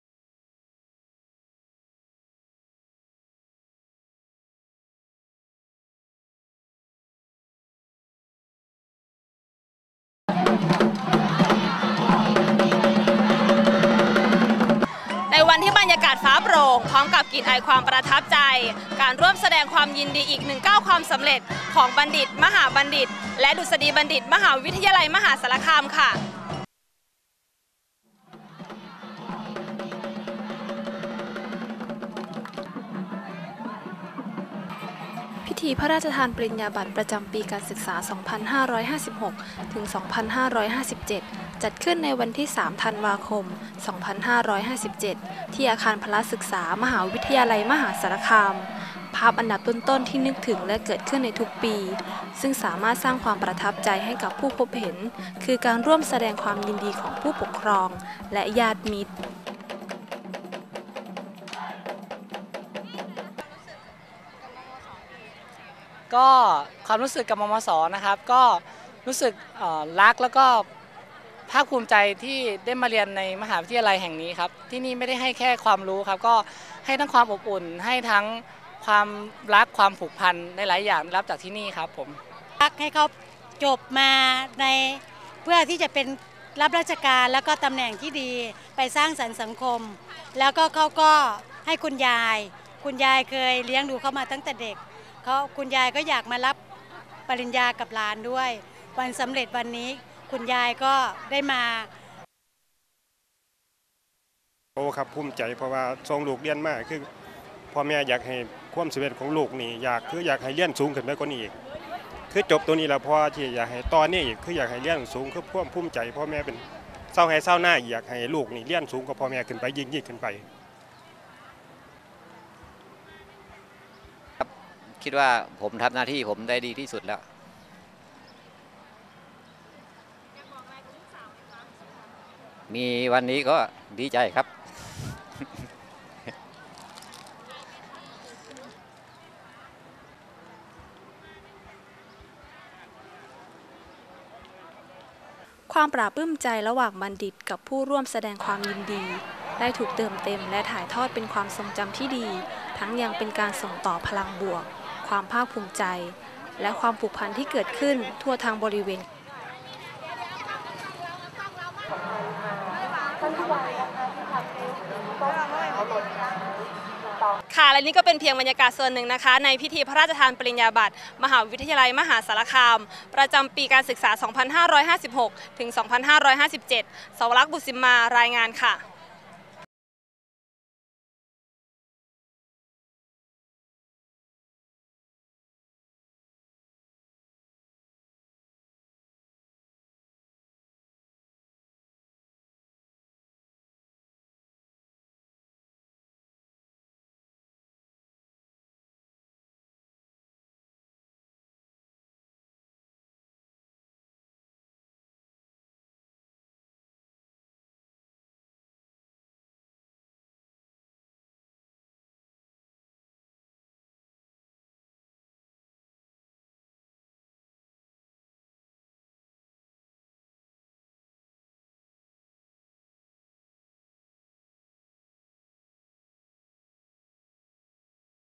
สกู๊ปพิเศษ : งานพิธีพระราชทานปริญญาบัตร มหาวิทยาลัยมหาสารคาม ประจำปีการศึกษา 2556-2557
วิดีโอ / ตัดต่อ :  เสาวลักษณ์ บุตรสิมมา